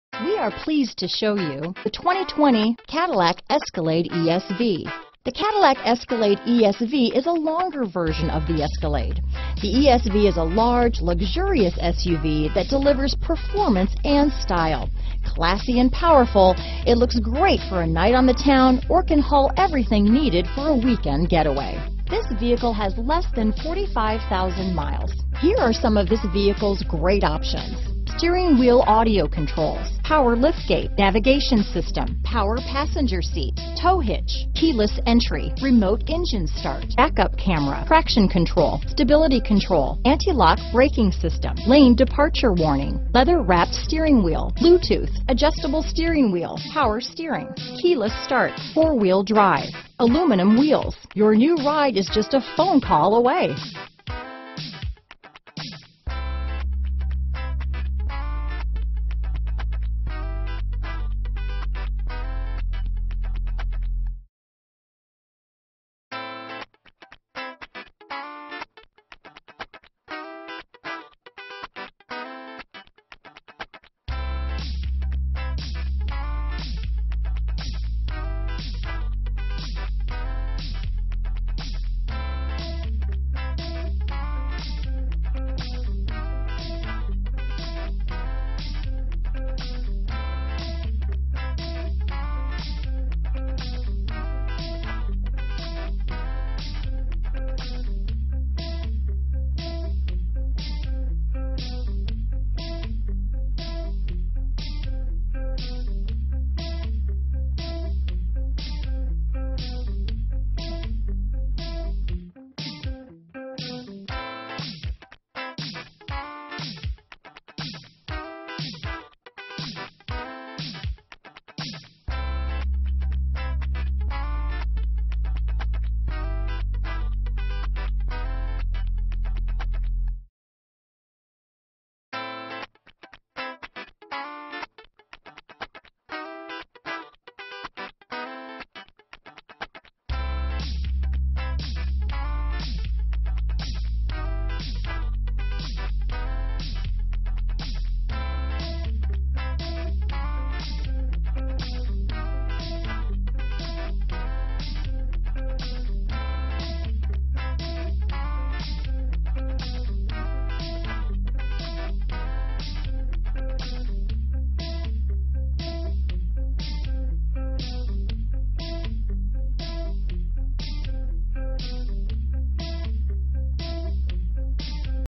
2020 Cadillac Escalade ESV near me Highland Park, Arlington Heights, Skokie, Libertyville, Glenview,
Black Raven Used 2020 Cadillac Escalade ESV available near me in HIghland Park, Illinois at Highland Park Ford. Servicing the Arlington Heights, Skokie, Libertyville, Glenview, IL area.
2020 Cadillac Escalade ESV Luxury - Stock#: 43766 - VIN#: 1GYS4HKJ5LR115026

CADILLAC USER EXPERIENCE WITH EMBEDDED NAVIGATION  AM/FM stereo with 8 diagonal color information display featuring touch response  haptic feedback  gesture recognition  Natural Voice Recognition  Phone Integration for Apple CarPlay and Android Auto capability for compatible phone  Connected Apps and Teen Driver.